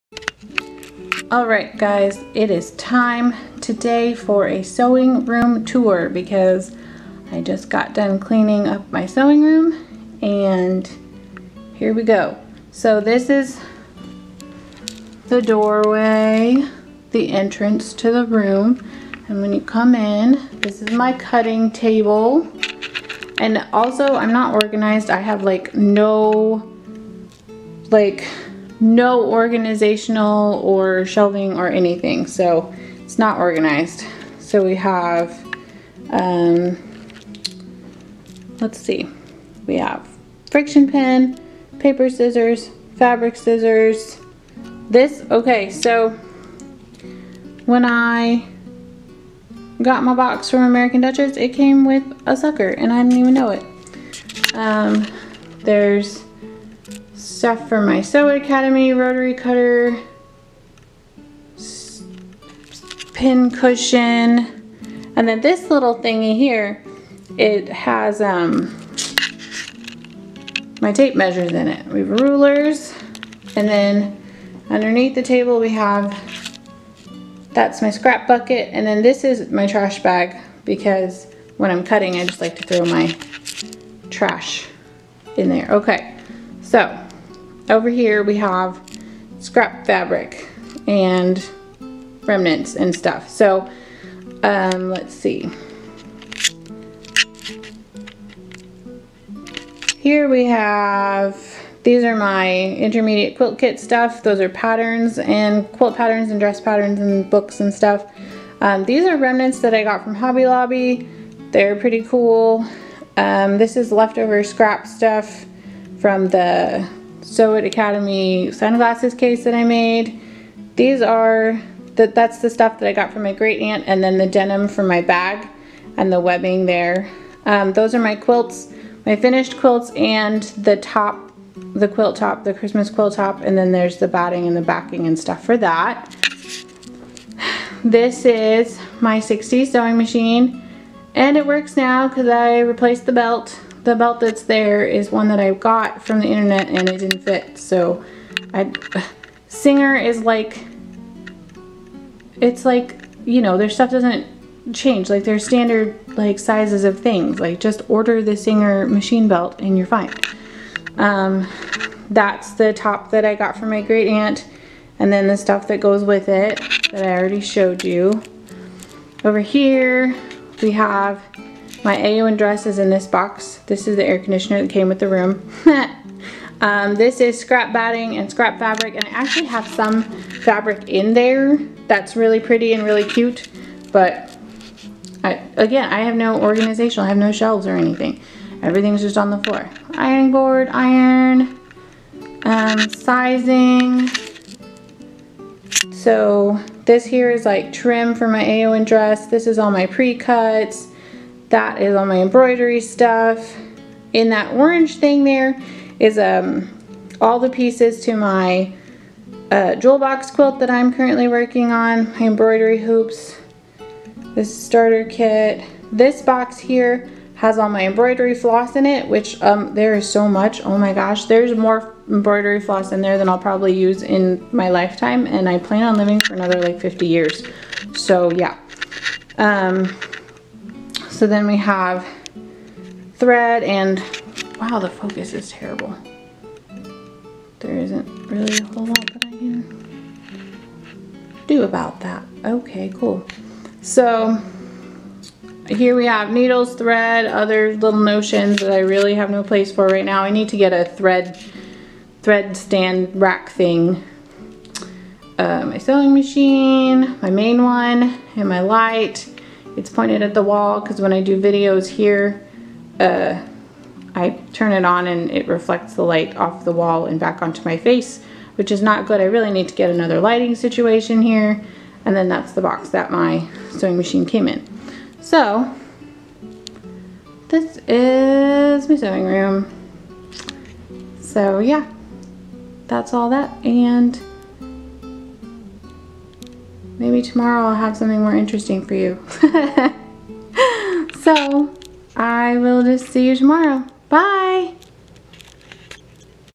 30-Day YouTube Challenge - Day 23: Sewing Room Tour
I cleaned up my sewing room today, so here's a tour. If anyone has any organizational tips, please let me know!